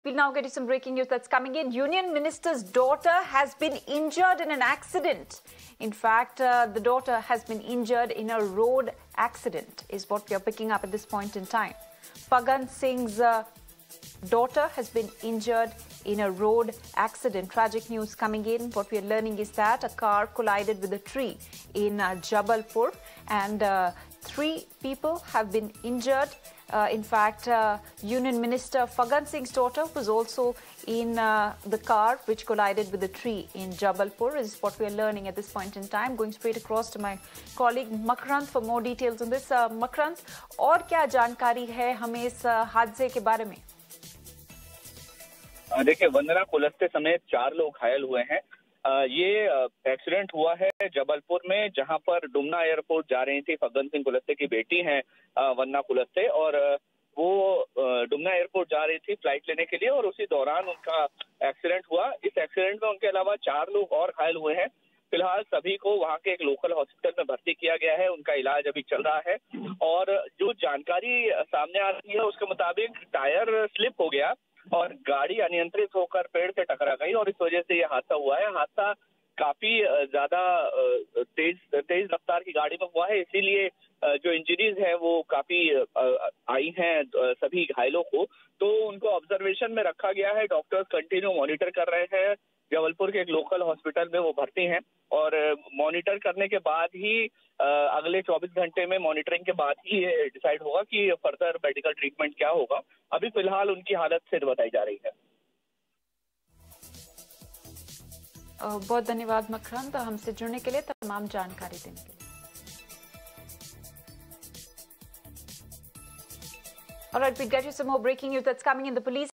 Breaking News: Union Minister's Daughter Injured In an Accident | Car Collides With Tree
Breaking News: Union Minister Faggan Singh Kulaste's daughter Vandana has been injured in a road accident. The car collided with a tree  and along with Vandana 3 more people has been injured. The incident took place at Jabalpur in Madhya Pradesh. Vandana was travelling to the Dumna airport when this incident took place.According to the information, the Scorpio car of Union Minister Kulaste's daughter Vandana Kulaste met with an accident near Banjari Mata Temple on Dumna Road in Jabalpur. All the three injured persons have been admitted to a private hospital in Jabalpur.